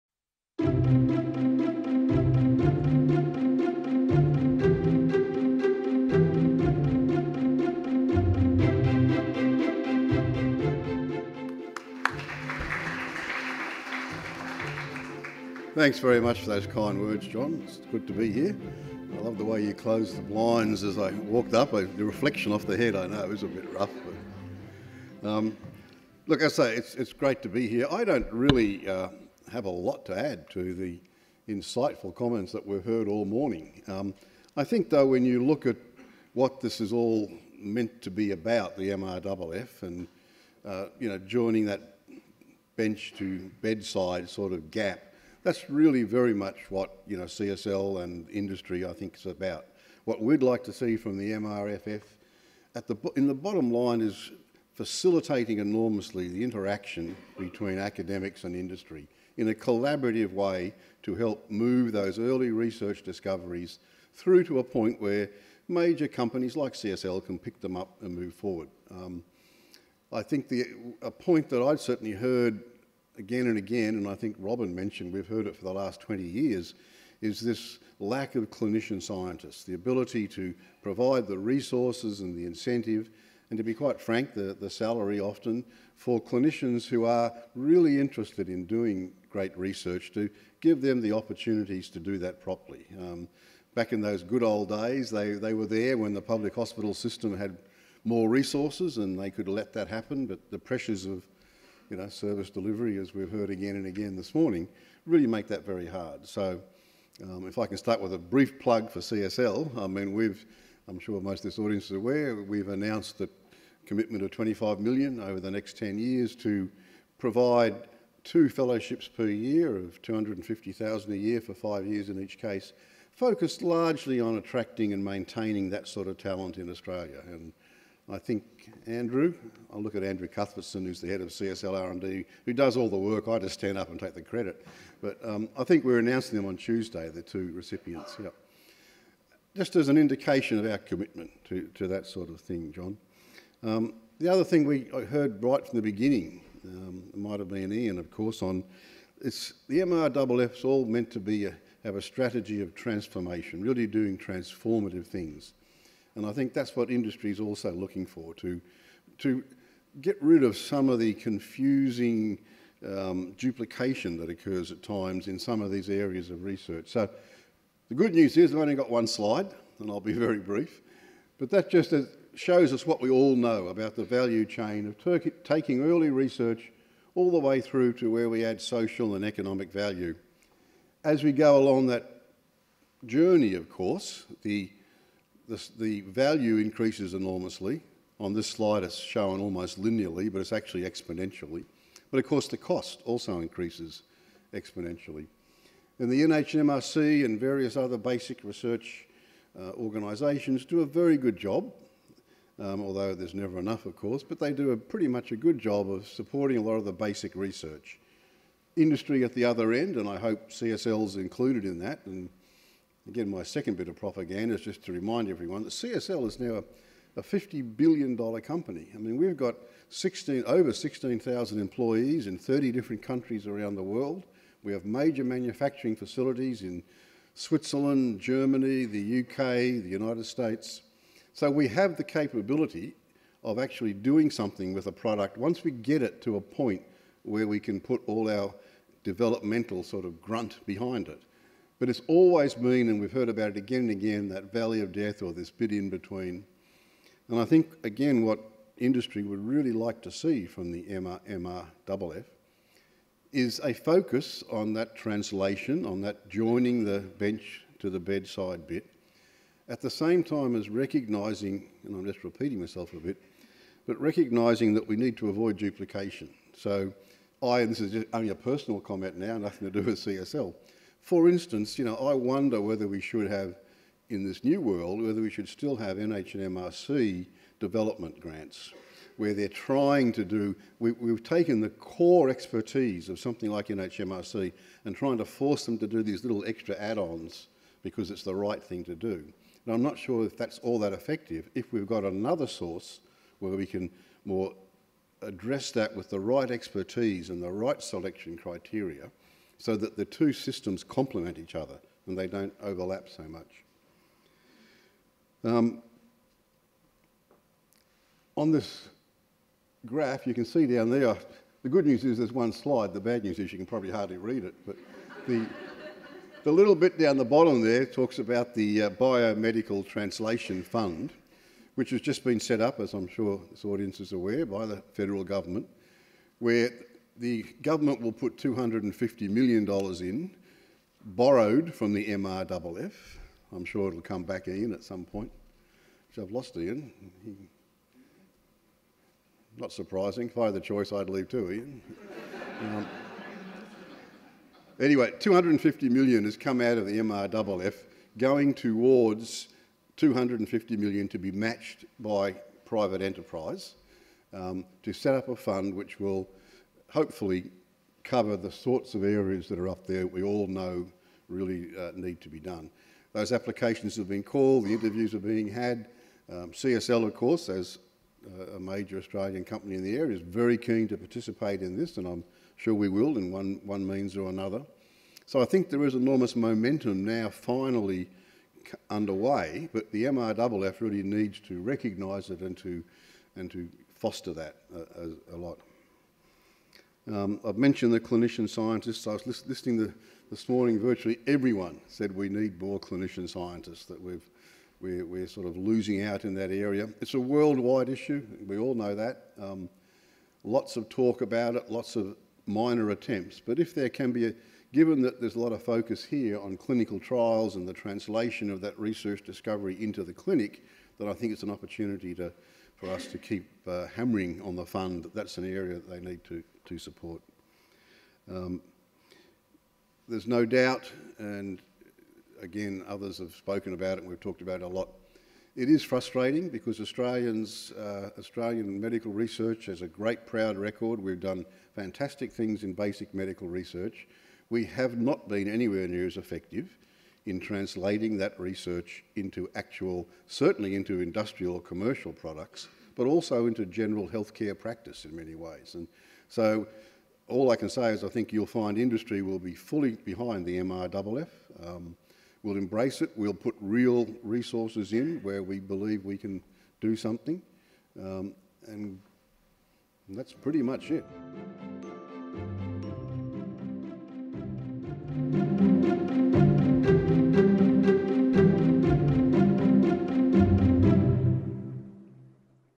Prof John Shine_AAHMS
The Medical Research Future Fund – Great expectations and sober realities. Will the MRFF help link bench and bedside?

Professor John Shine  Professor of Medicine and Molecular Biology, Garvan Institute of Medical Research, UNSW; Chairman, CSL Limited

What will the biotech and the commercialization sector be expecting from the MRFF?